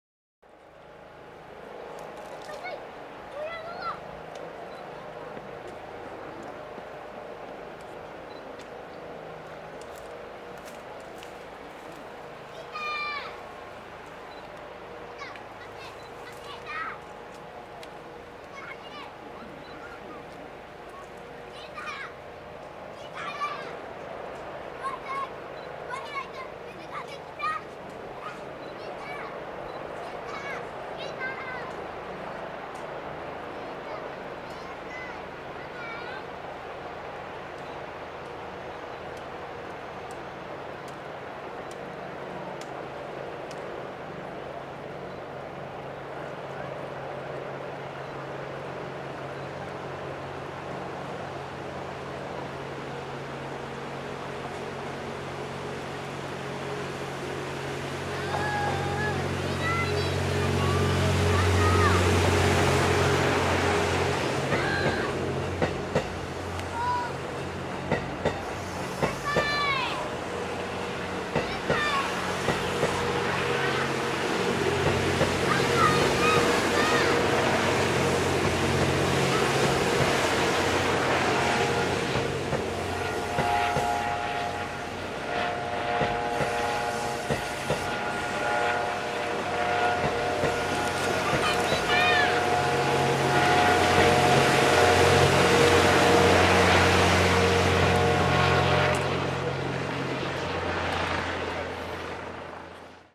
[4K60P] 餘部橋梁を渡るトワイライトエクスプレス瑞風一番列車 Panasonic GH5
２０１７年６月１７日に運行を開始したトワイライトエクスプレス瑞風。
その初列車が餘部橋梁を渡るシーンです。
Panasonic GH5+LUMIX G VARIO 14-42mm / F3.5-5.6 II ASPH.